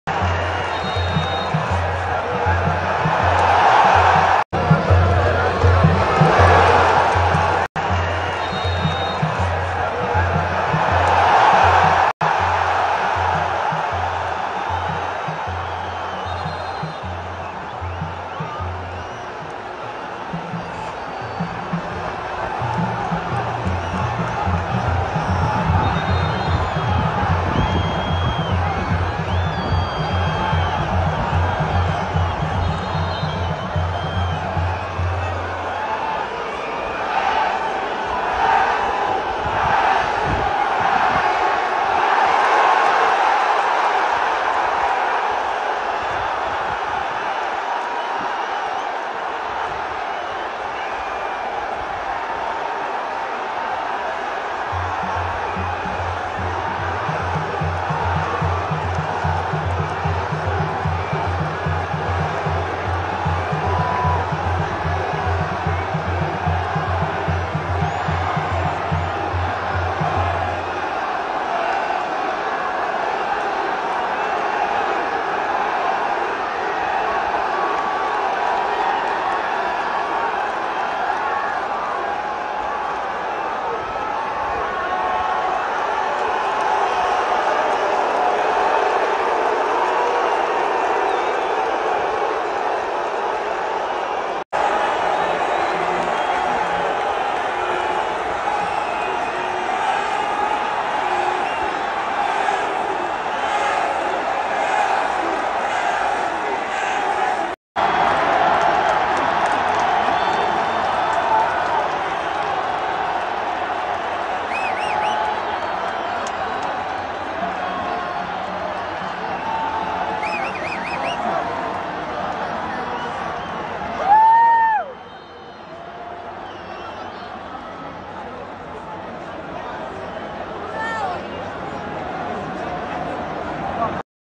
Cristiano Ronaldo Red Card vs Al Hilal Almost Hits Referee
Watch Ronaldo Red Card vs Al Hilal